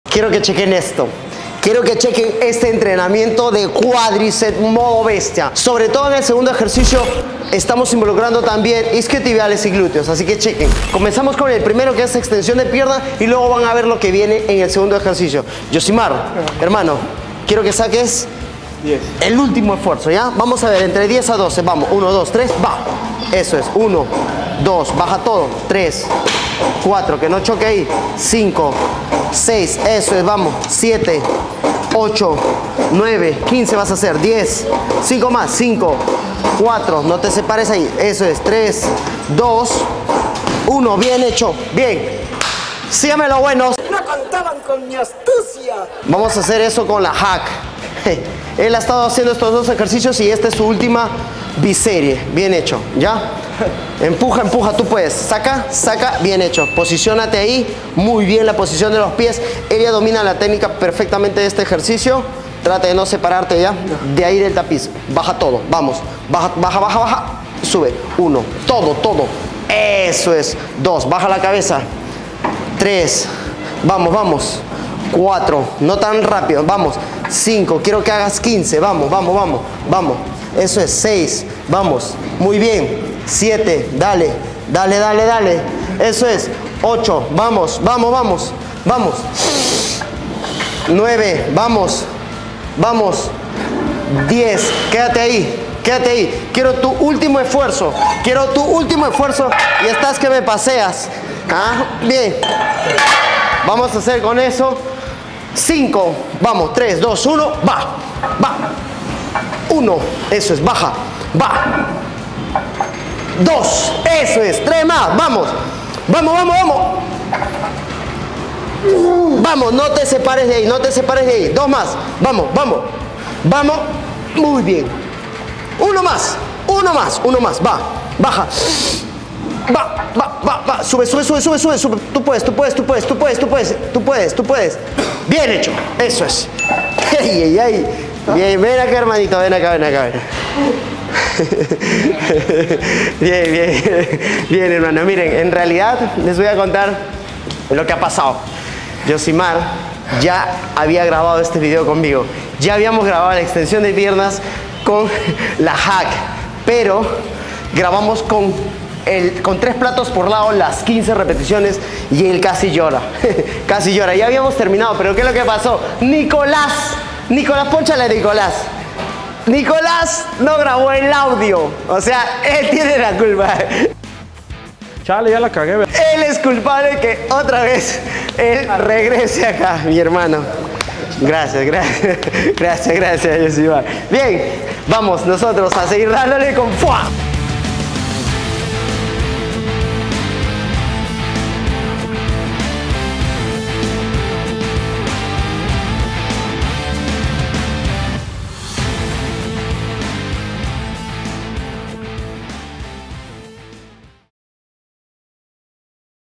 QUERIA ENTRENAR PIERNAS ¡TERMINO LLORANDO! | Frank Serapión.
¿Podrás hacerlo? 🥵🦵

Gimnasio: REEVES GYM
Ciudad: LIMA - PERÚ
Tour Perú Entrena con FUA🔥